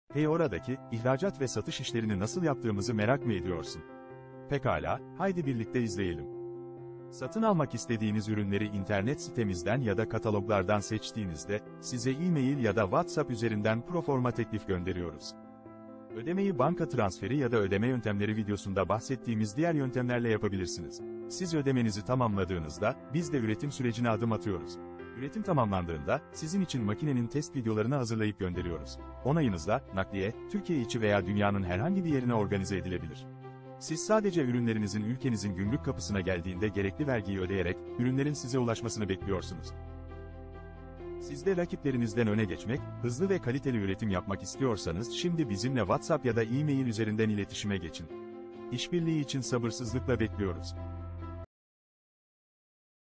İhracat ve Satış İşlemleri: nasıl kolaylaştırıyoruz!
Cengizler Ferforje olarak, ihracat ve satış işlemlerini kolaylaştırıyoruz. Ürünleri seçin, ödeme yapın, teslimat alın ve ihtiyacınıza uygun makinelerin tadını çıkarın. İhtiyacınız olduğunda kurulum ve servis desteği sağlıyoruz. İhracat belgeleri konusunda da size yardımcı oluyoruz.